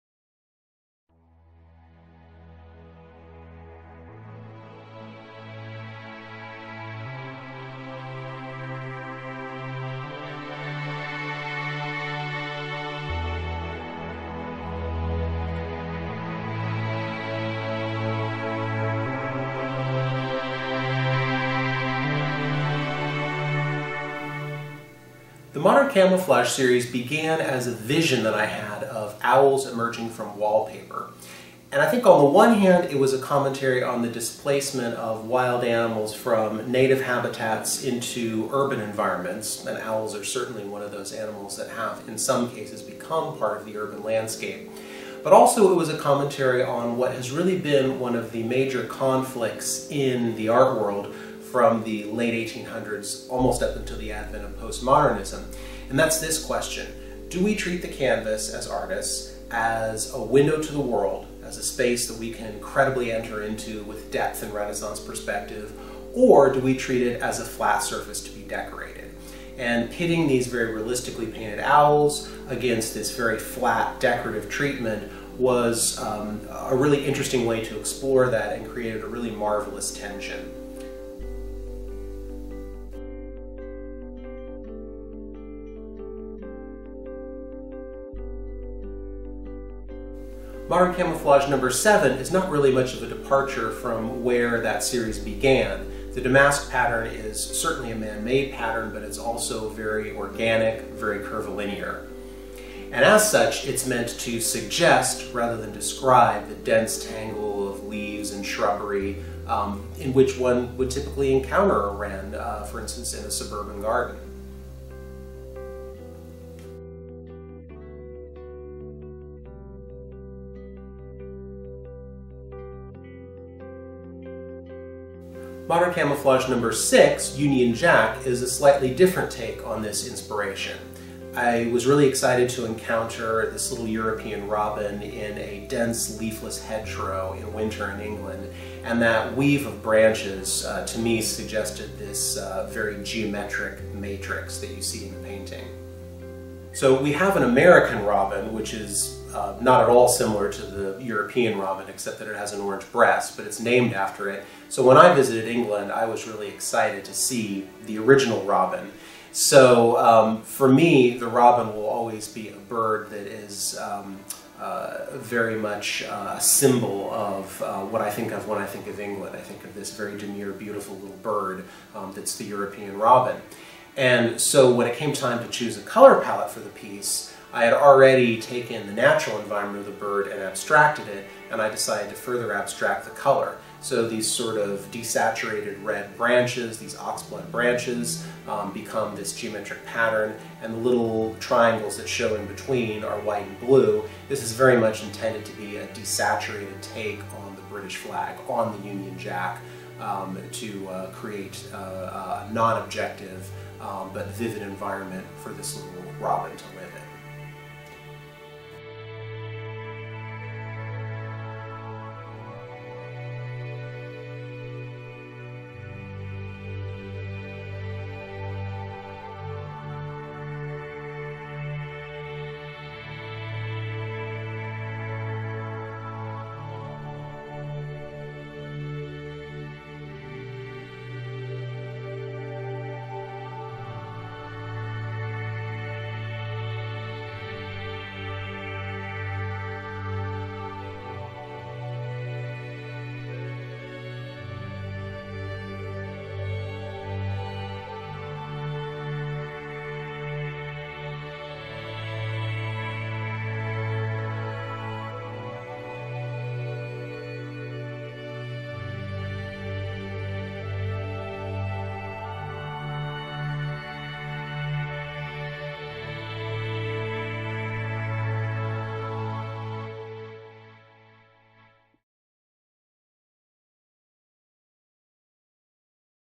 A Conversation with the Artist, Episode #1: About Modern Camouflage
Artist Andrew Denman speaks candidly about the inspiration and thought process behind his "Modern Camouflage" series of paintings.   This is Part One of a four part series produced in conjunction with Denman's two-man exhibition with sculptor Simon Gudgeon, "Pattern & Poise," opening in November of 2018 at Sculpture by the Lakes, Dorset, England.